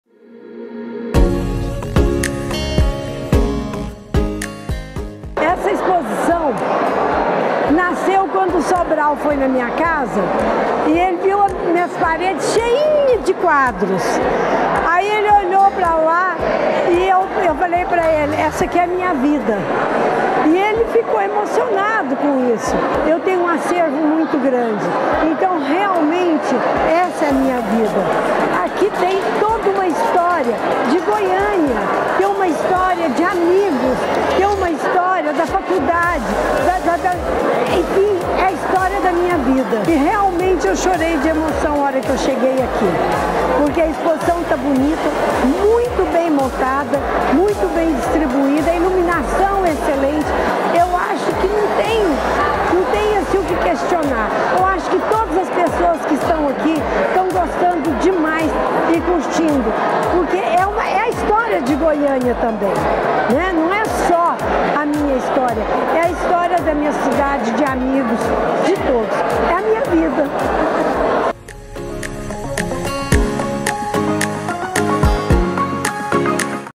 EXPOSIÇÃO É A MINHA VIDA SÁIDA CUNHA
“Minha vida está aqui”, de Sáida Cunha está sendo um sucesso aqui no Octo. A entrada para conferir é gratuita e a exposição ficará disponível até 5/11. Não perca tempo e aprecie. ✨